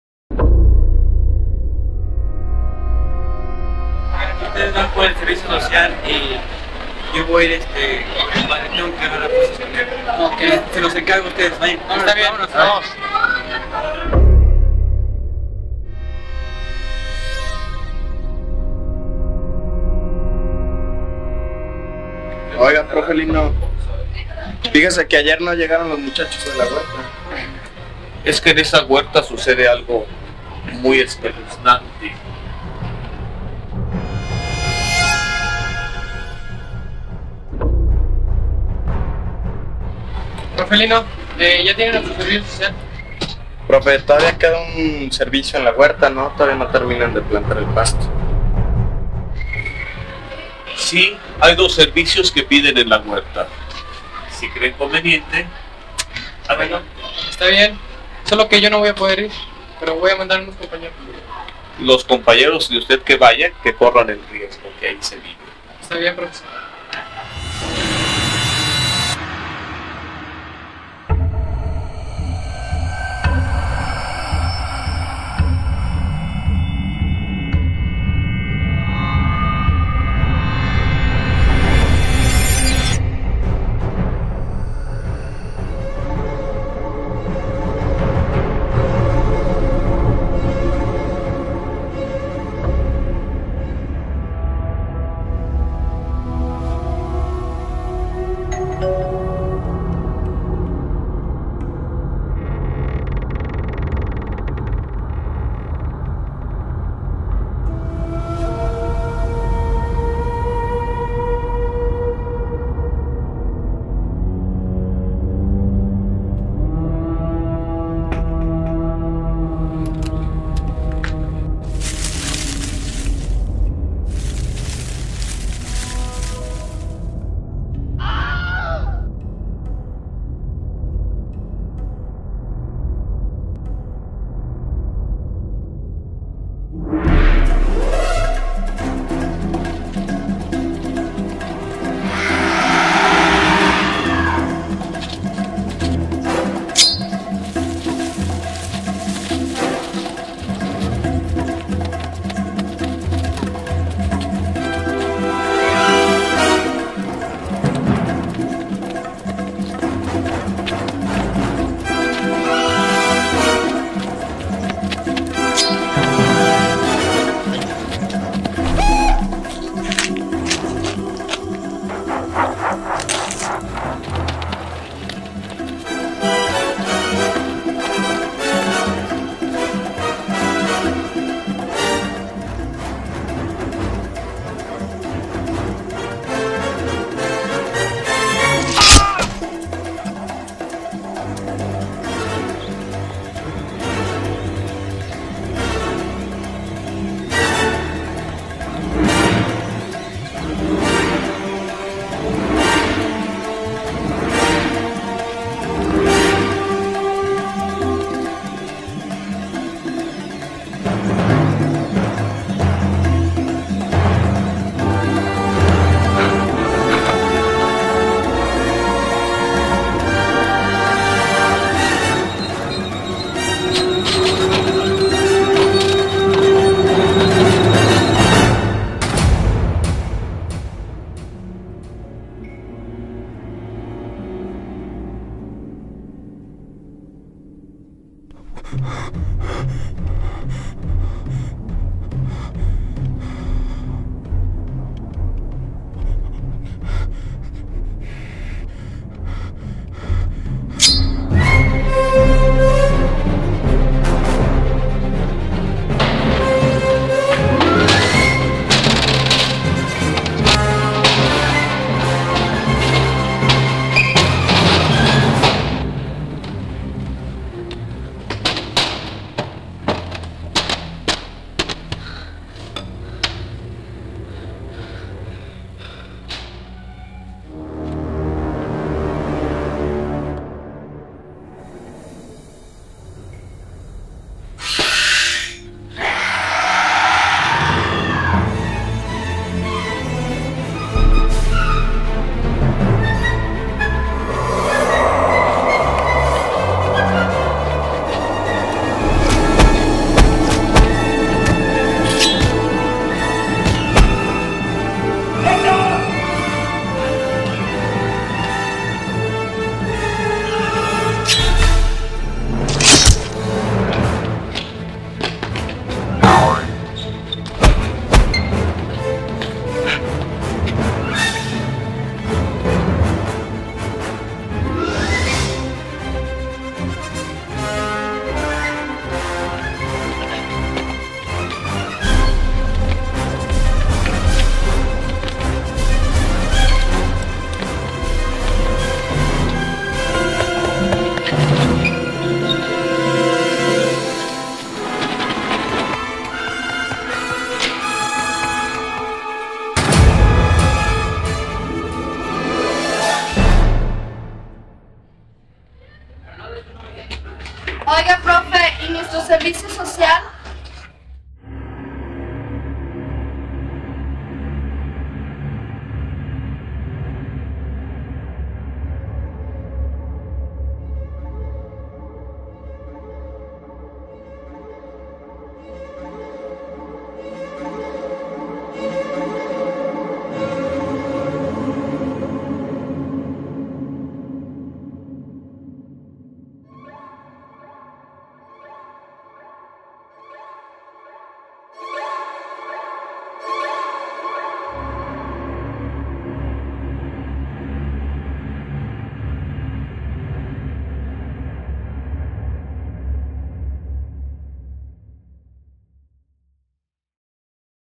TERROR EN EL SERVICIO SOCIAL - Shortfilm 1998
Cuatro estudiantes de Bachillerato son enviados a una huerta, a realizar su servicio social. Lo que no saben es que en ese lugar quedarán encerrados por una secta maldita, que destaza y se come a sus víctimas.

Este fué mi primer cortometraje de ficción hecho en 1998, no tenía conocimiento didáctico ni técnica. Pero con muchas ganas de contar una historia de persecución y acción, pasé de dibujar cómics con esas aventuras a aventurarme  junto con mis amigos del salón a realizarlas en forma de cortometraje.
Después del resultado final, descubrí todo un mundo fascinante de como contar historias.

Cortometraje hecho con fínes de entretenimiento.